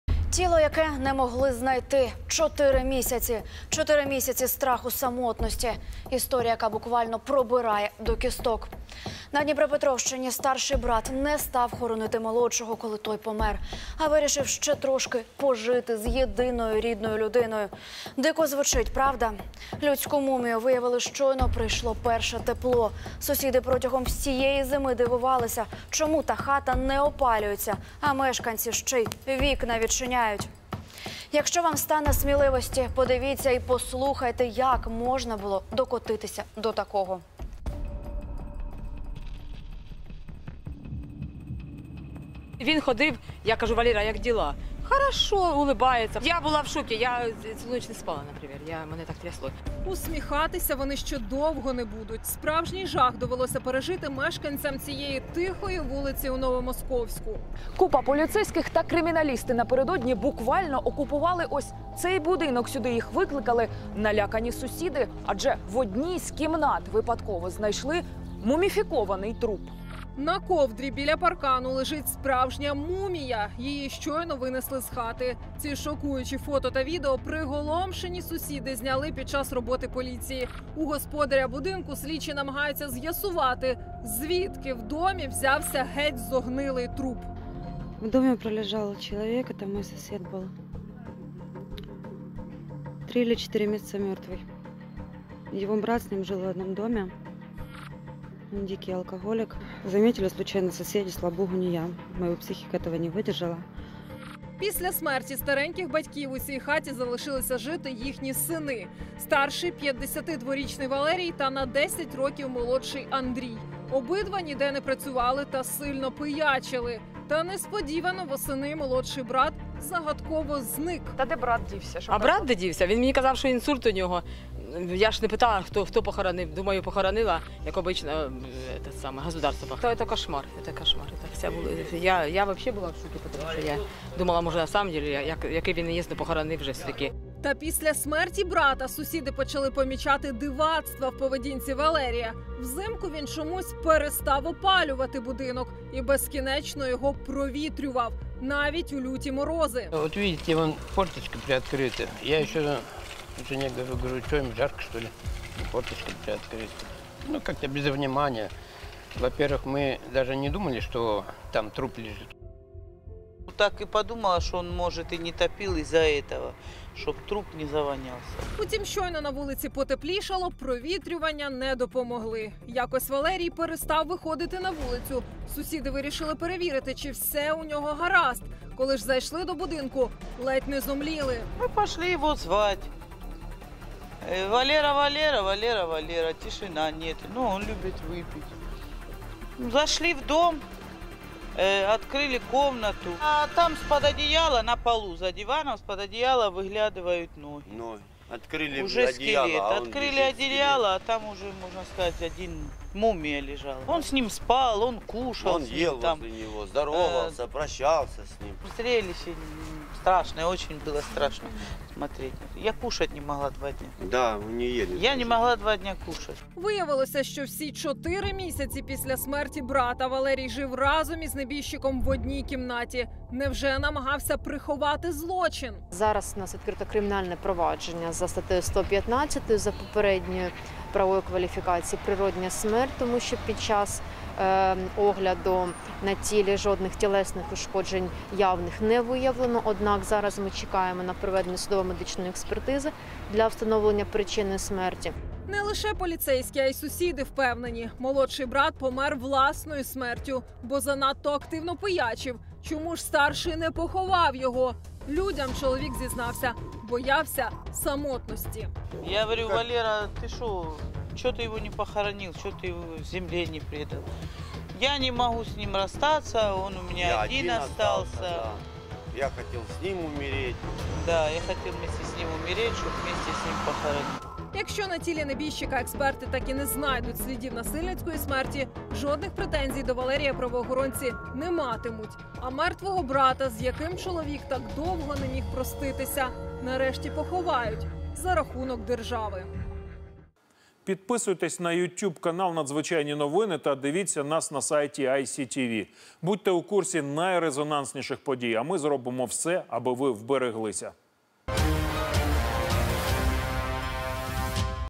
Як воно - жити з трупом брата? Справжня мумія українця за диваном у сусіда
Понад чотири місяці жив із трупом власного брата у одній кімнаті, бо боявся самотності.

Дика історія із Дніпропетровщини.

У Новомосковську в одному із будинків люди виявили жахливу знахідку – справжню людську мумію за диваном свого 52-річного сусіда.

Ще минулої осені зненацька помер його молодший брат.  []

Сусідам старший сказав, що самотужки поховав небіжчика.

Але чомусь цілу зиму навіть у люті морози підозріло перестав опалювати будинок і нескінченно його провітрював.

Моторошна правда відкрилась, щойно настала відлига.

Провітрювання - не допомогли.

Лише подивіться, який жах довелось побачити сусідам.